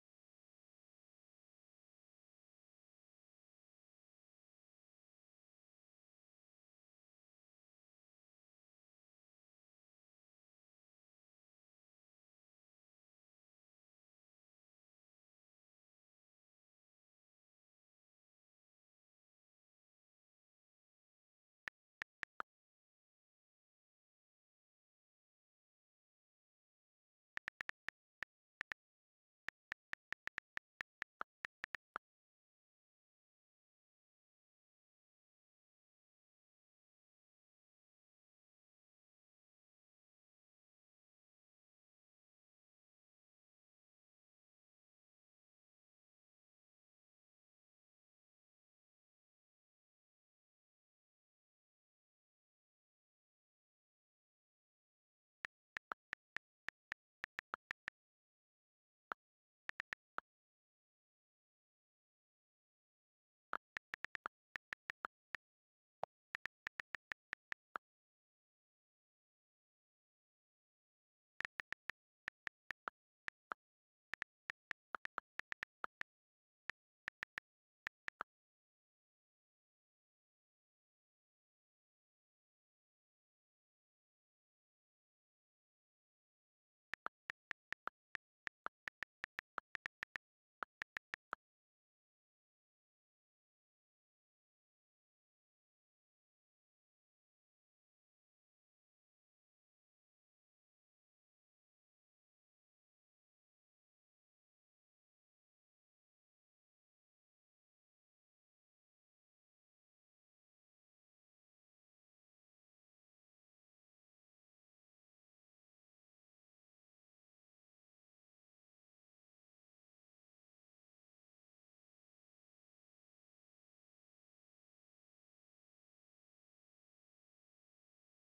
proof of me getting abyssal hunter bt
i used a heavenly 2 fortune 1 basic blessing huge fortune 1 and lucky potion
with exo and jackpot